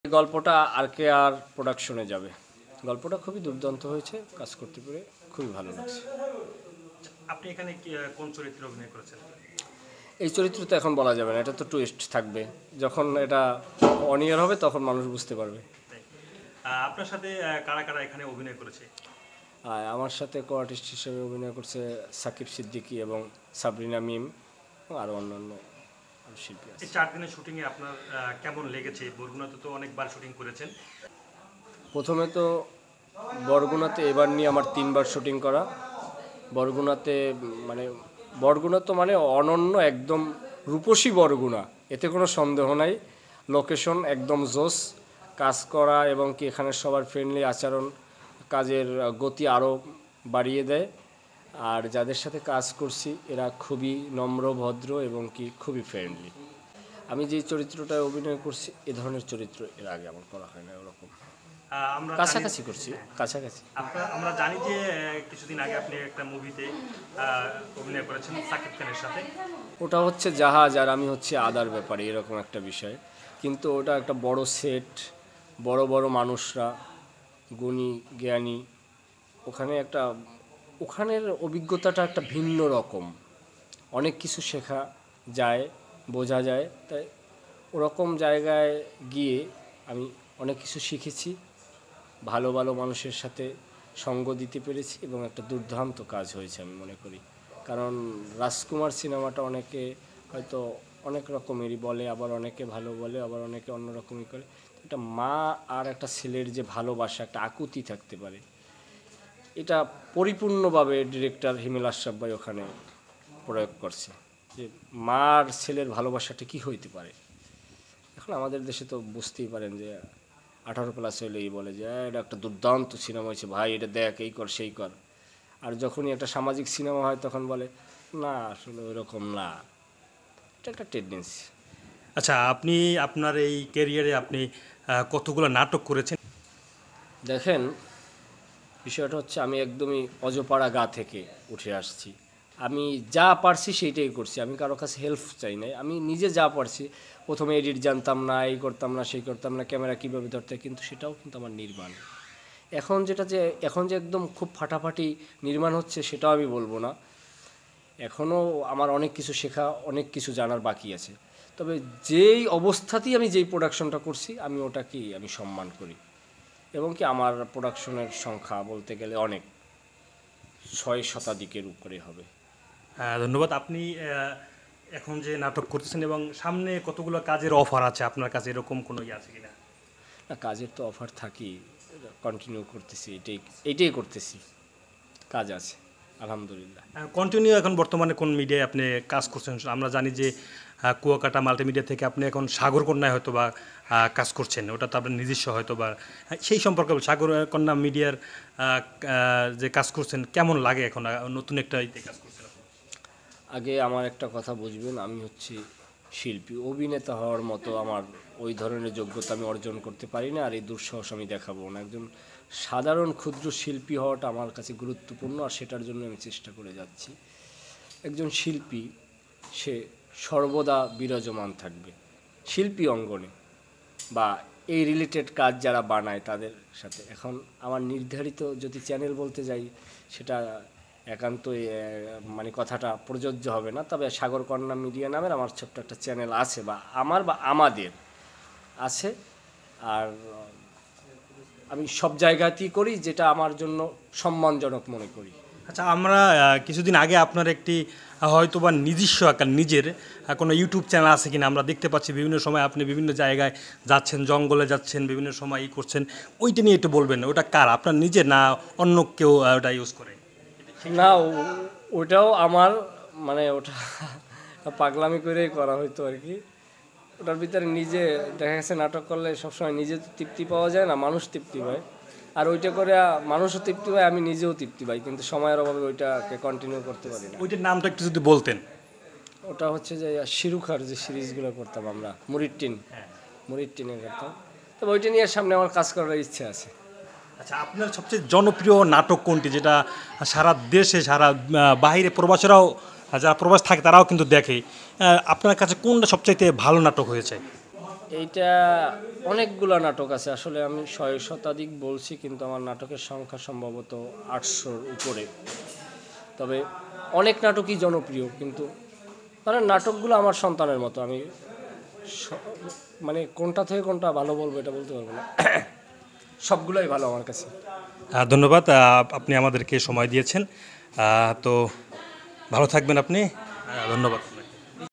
সাদ্দাম মাল, নতুন নাটক সিনেমা নিয়ে যা বললেন
সাদ্দাম মাল ও সাকিব সিদ্দিকির নতুন ধামাকা নাটক দেখবেন RKR_Production ইউটিউব চ্যানেলে

L I K E __ C O M M E N T __ S H A R E __ S U B S C R I B E __ LOVE YOU ALL
Vlogs
Bangladesh
news